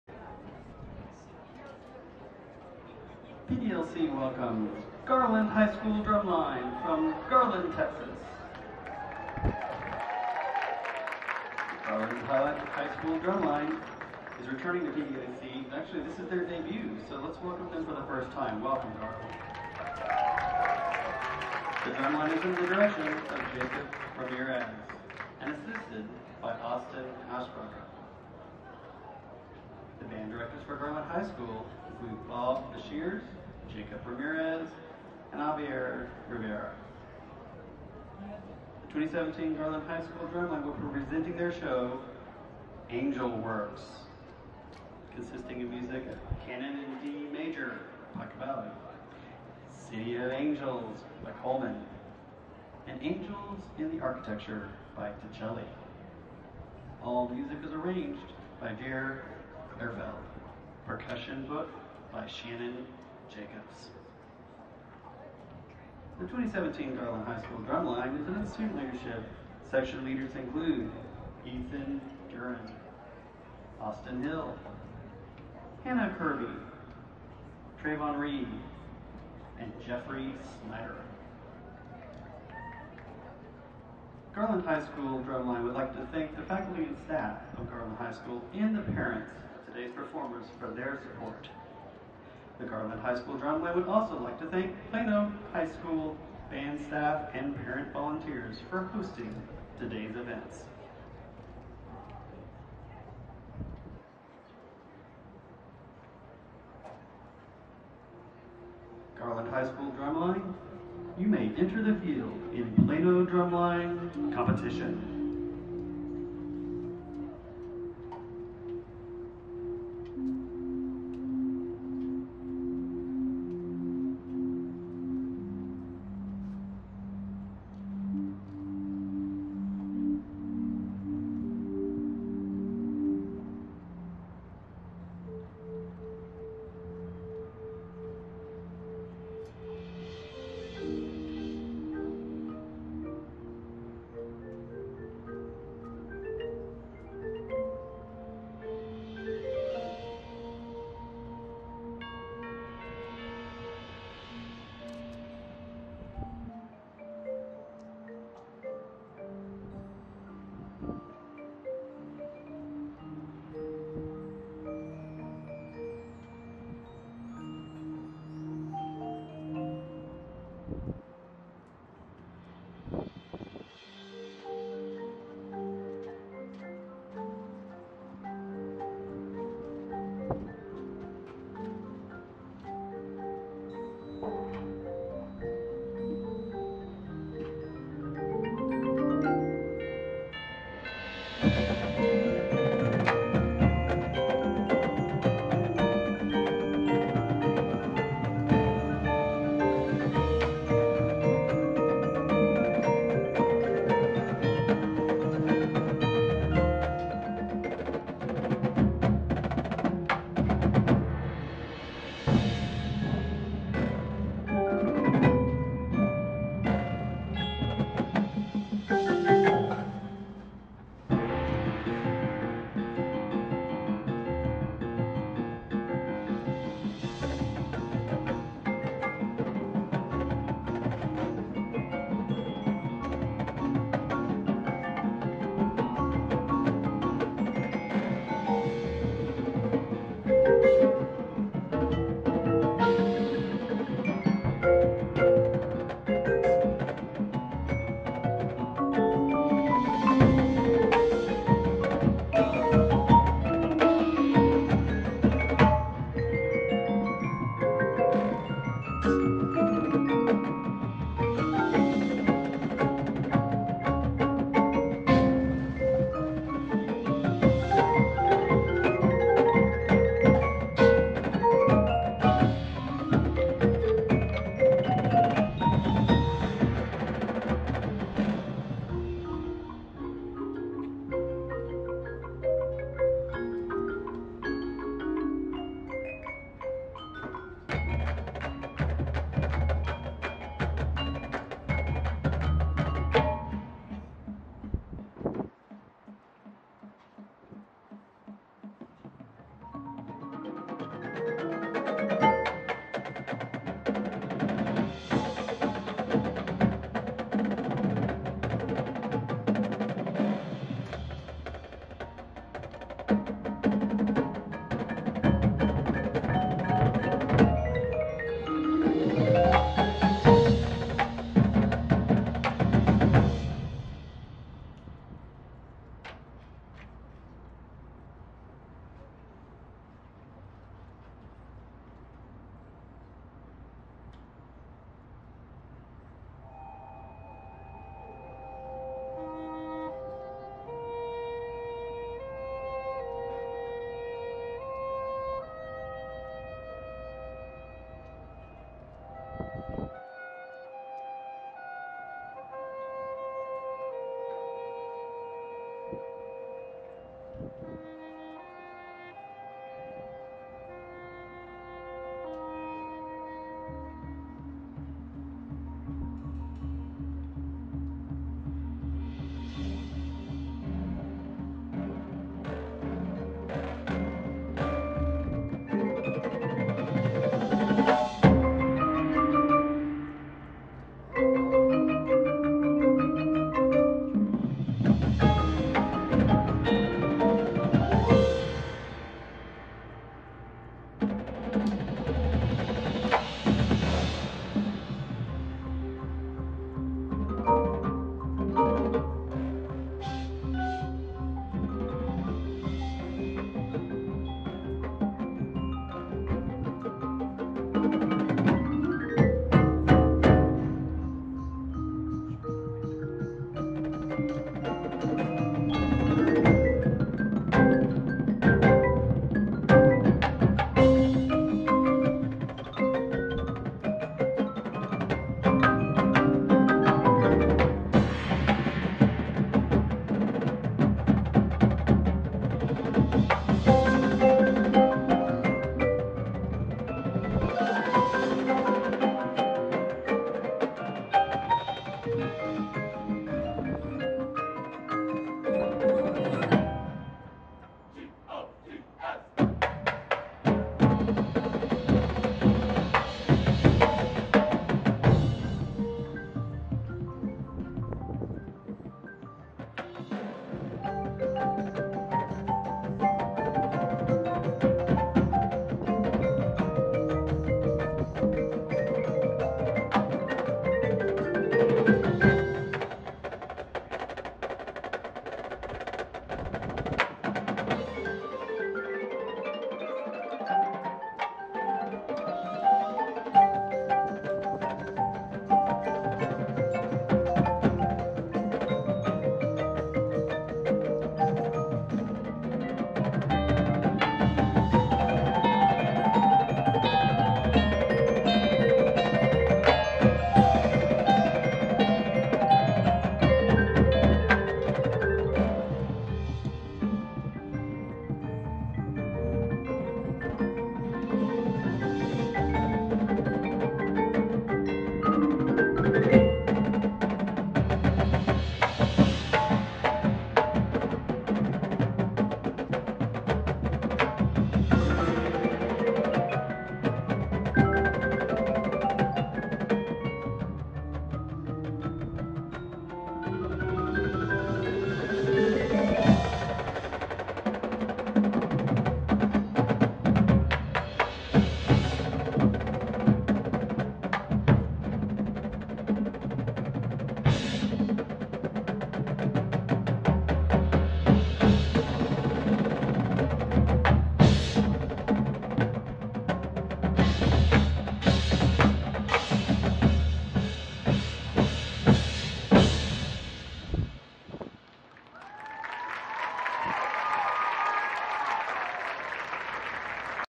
Garland High School Drumline @ Plano Drumline Competition 2017
The G.O.D.S. received 2nd in the A Standstill Division II with a score of 250/300 and tied for 7th overall out of 54 groups. We were considered for all captions we were eligible for. Performance on 9/23/17.